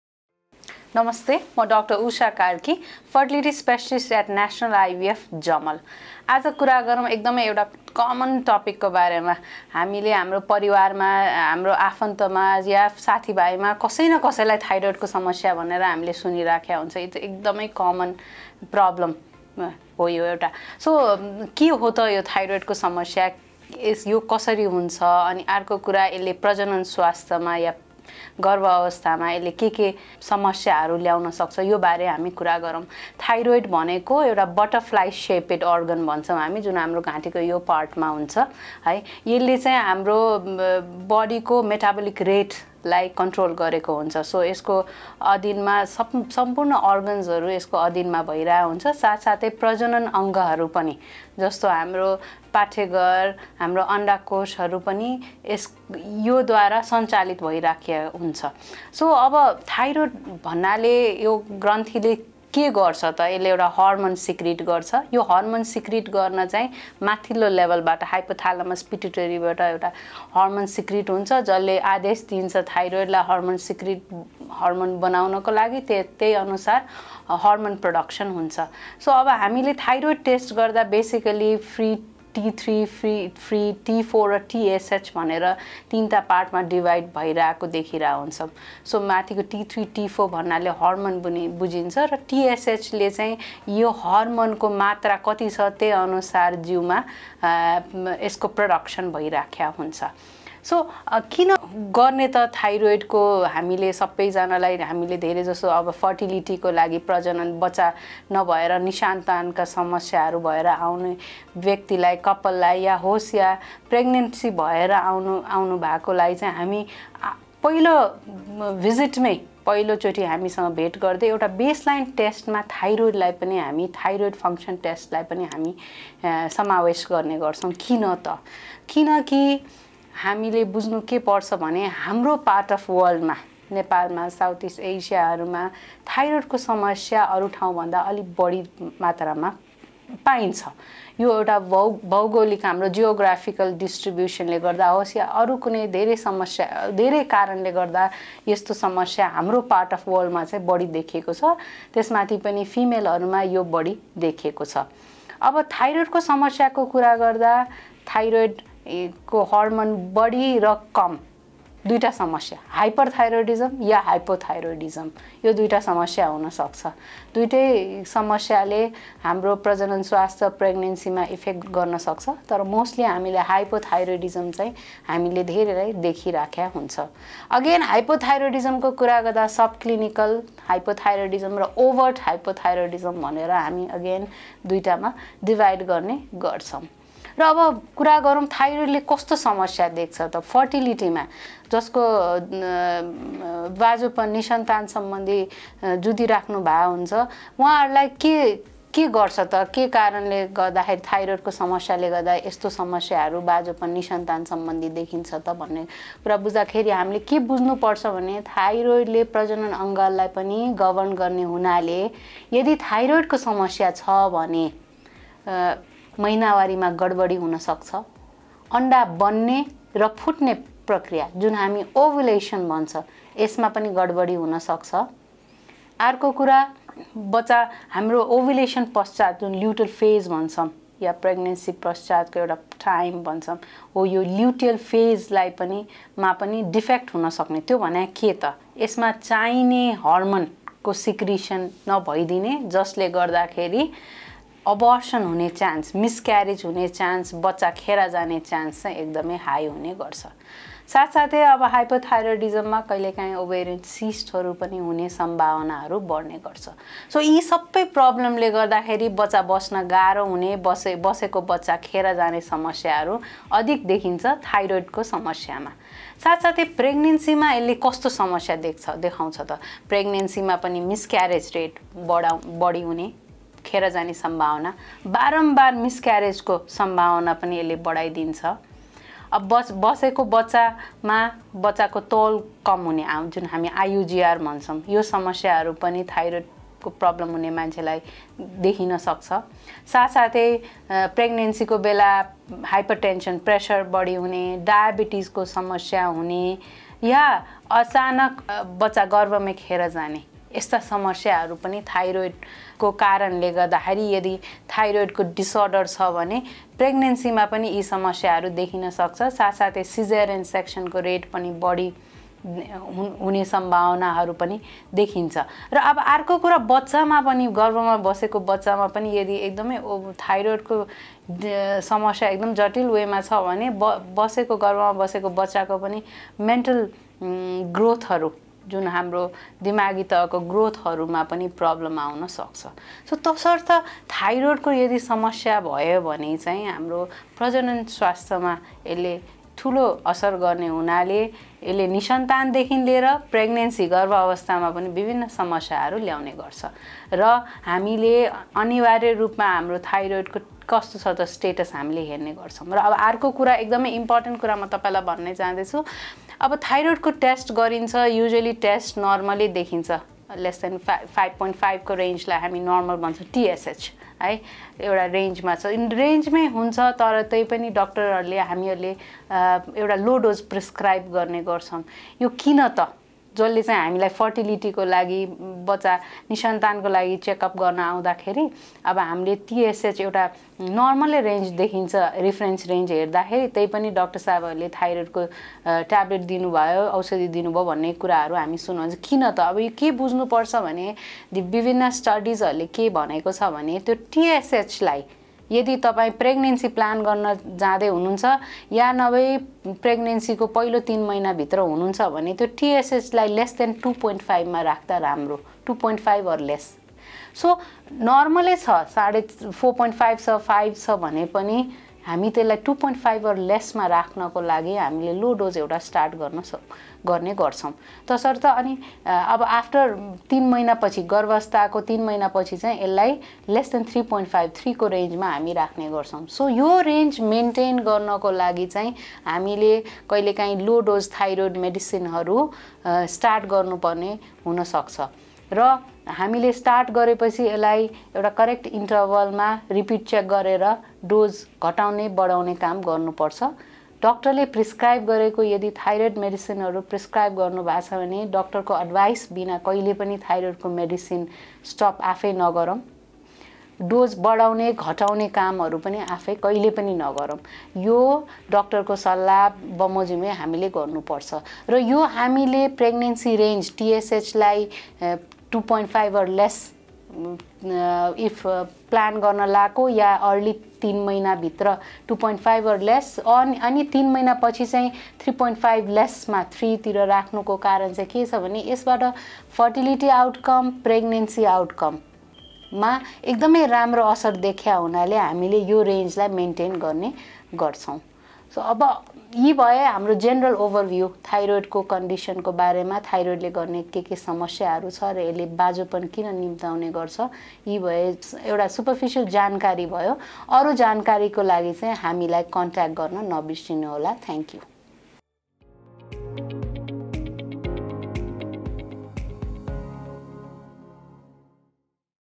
Is there Relationship between Thyroid Disorder and Infertility?
Thyroid Disorder can significantly affect Reproductive Health. In this video, we explore the science behind how thyroid disorders, such as hypothyroidism and hyperthyroidism, can affect fertility .Discover the crucial role thyroid hormones play in conception and pregnancy, and gain insights into potential challenges that individuals with thyroid disorders may face on their journey to parenthood.

Location : Jyoti Bhawan, 4th Floor, Kantipath, Jamal , Kathmandu, Nepal